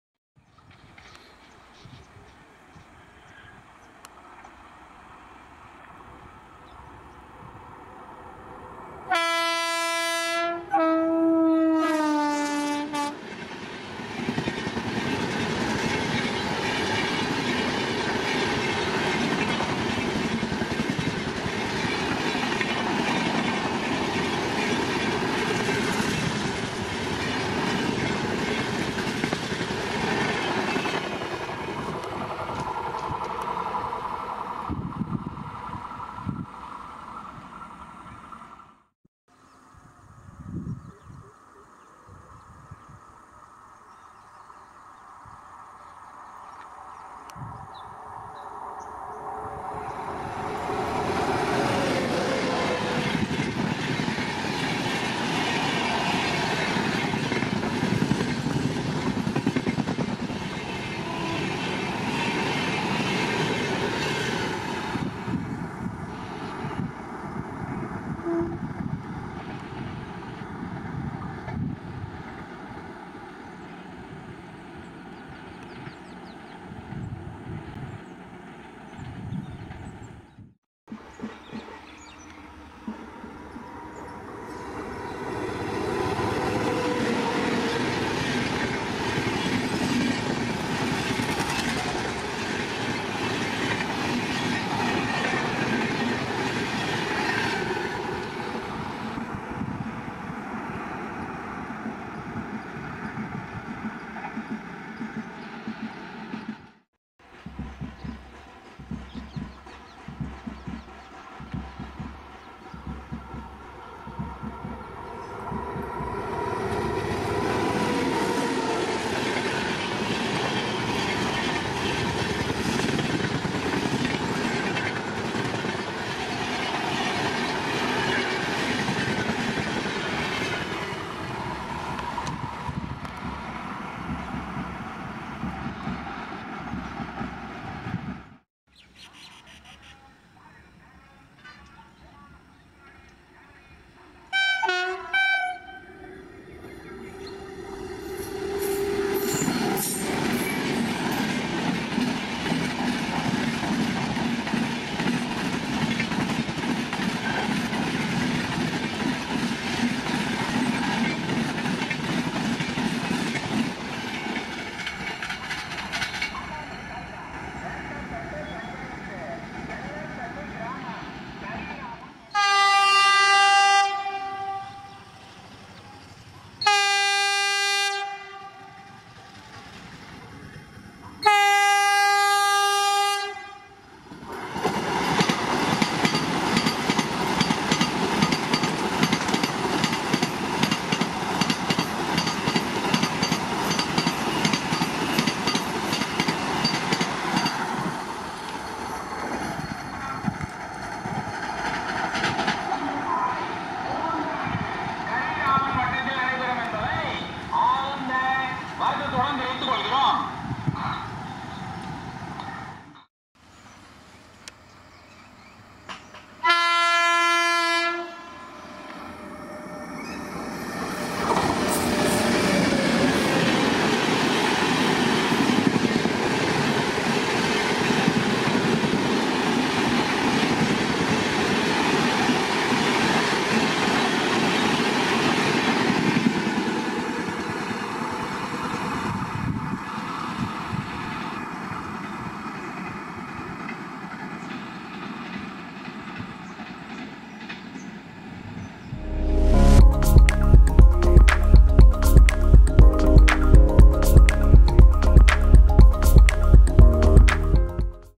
Non-stop Premium trains of South India : Shatabdi, Double Decker Gang, Vande Bharat, Duronto, UDAY
Most Spectacular Premium High-Speed actions from South India ft. Shatabdi Gang, Double Decker, Uday Gang, Vande Bharat, Duronto with Beautiful Mountain Backdrop at full speed.

Sections: Chennai - Bangalore section
Coimbatore - Bangalore section
Location - Jolarpettai Junction [JTJ], Southern Railways

Trains featured in this video.

00:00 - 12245 Howrah - SMVT Bengaluru Duronto Express
12245 हावड़ा - सर एम. विश्वेश्वरय्या टर्मिनल बेंगलूरु दुरंतो एक्सप्रेस
Engine: SRC (Santragachi) HOG WAP-7i #37066

00:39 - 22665 KSR Bengaluru City Junction - Coimbatore Uday Express
22665 क्रा सं रा बेंगलुरु - कोयम्बत्तूर उदय एक्सप्रेस
Engine: RPM (Royapuram) HOG WAP-7i #37207 (Pranav Jewellery Vinyls with Lightning Bolts livery)

01:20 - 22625 MGR Chennai Central - KSR Bengaluru City AC Double Decker Express
22625 एम. जी. आर. चेन्नै सेन्ट्रल - क्रा सं रा बेंगलूरु वातानुकूलित डबल डेकर एक्सप्रेस
Engine : RPM (Royapuram) HOG WAP-7i #30680

01:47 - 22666 Coimbatore - KSR Bengaluru UDAY Express
22666 कोयम्बत्तूर - क्रा सं रा बेंगलुरु उदय एक्सप्रेस

02:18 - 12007 MGR Chennai Central - Mysuru Shatabdi Express
12007 एमजीआर चेन्नई सेंट्रल-मैसूर शताब्दी एक्सप्रेस
Engine : offlink KYN (Kalyan) HOG WAP-7i #30799

02:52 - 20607 MGR Chennai Central - Mysuru Vande Bharat Express
20607 एमजीआर चेन्नई सेंट्रल-मैसूर वंदे भारत एक्सप्रेस

03:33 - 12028 KSR Bengaluru City - MGR Chennai Central Chennai Shatabdi Express
12028 क्रा सं रा बेंगलुरु सिटी -एम. जी. आर.चेन्नै सेन्ट्रल शताब्दी एक्सप्रेस
Engine : RPM (Royapuram) WAP-7i #30342

04:09 - Like, Comment, Share & Subscribe!